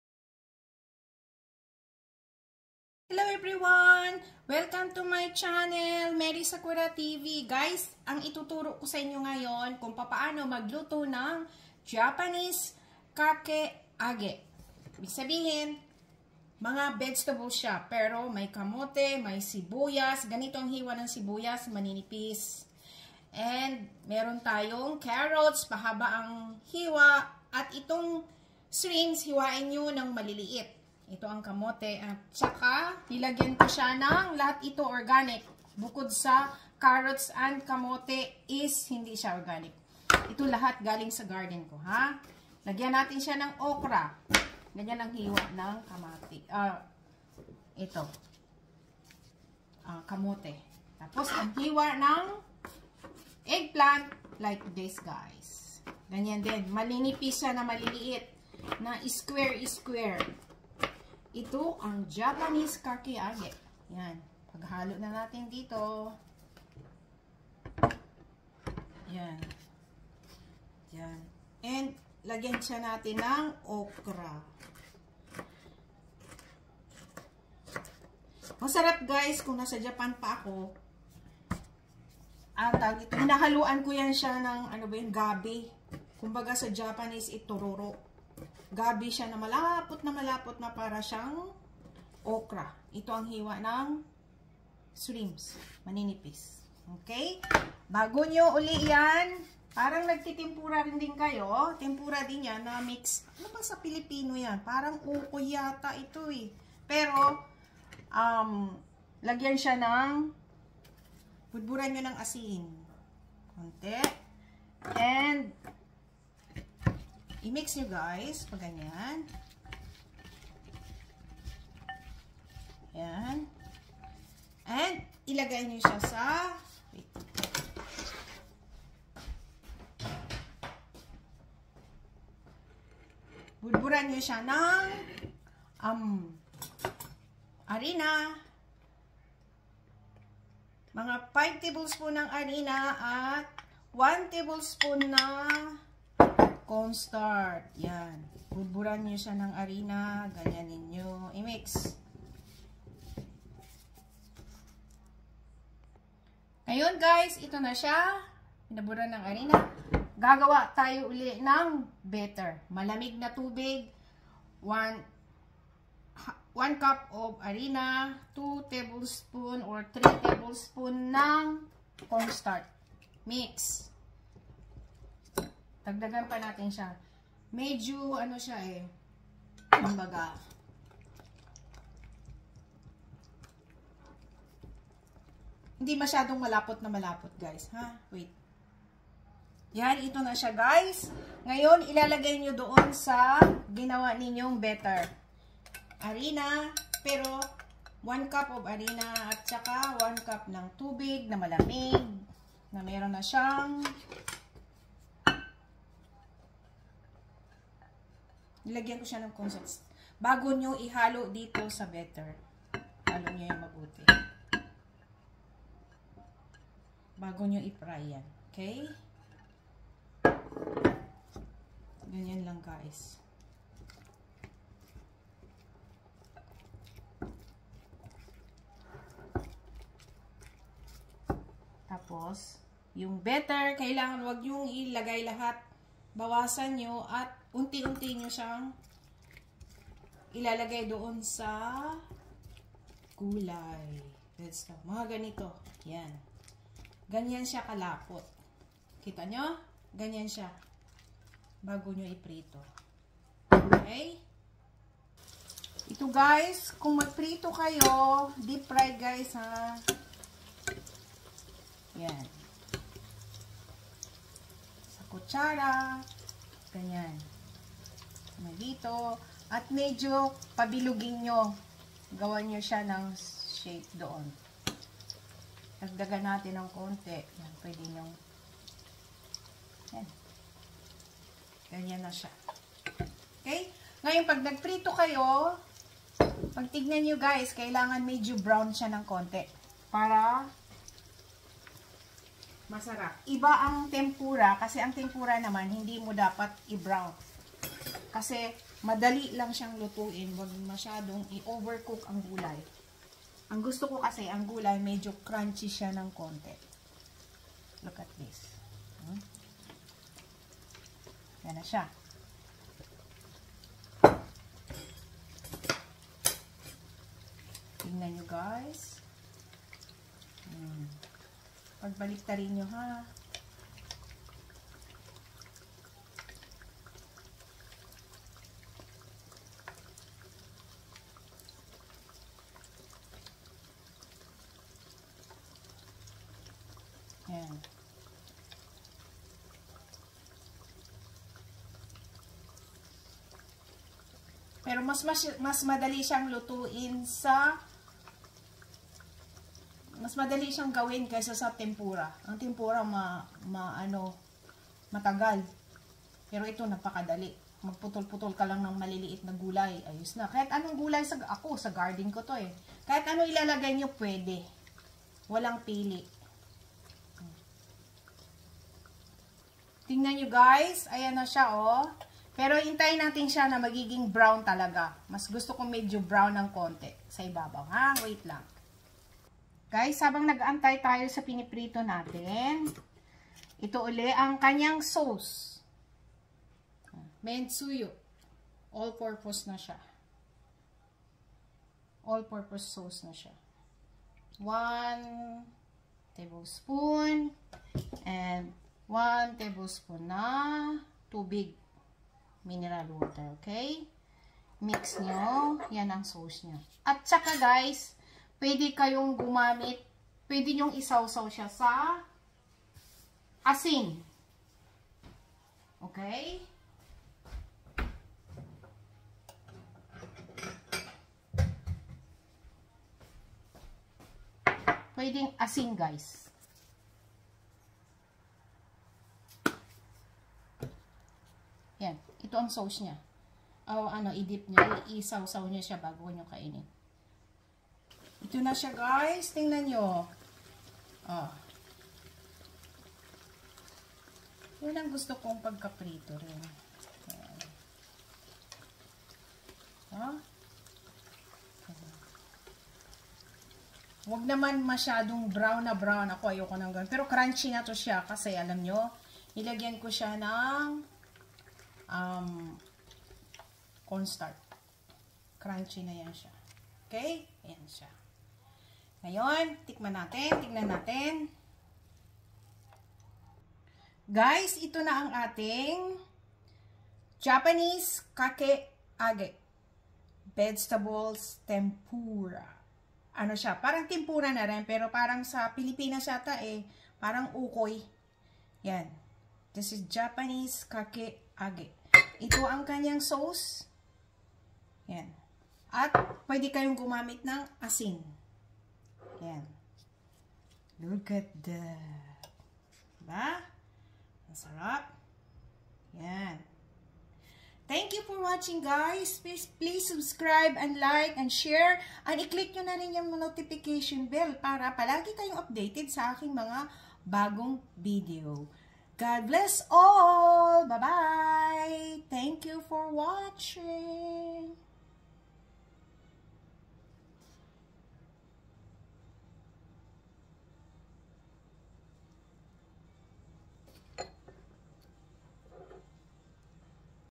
how to cook japanese kakeage, mix veges.fry,
Hi guys my vedio is how to cook japanese food
MERRY SAKURA TV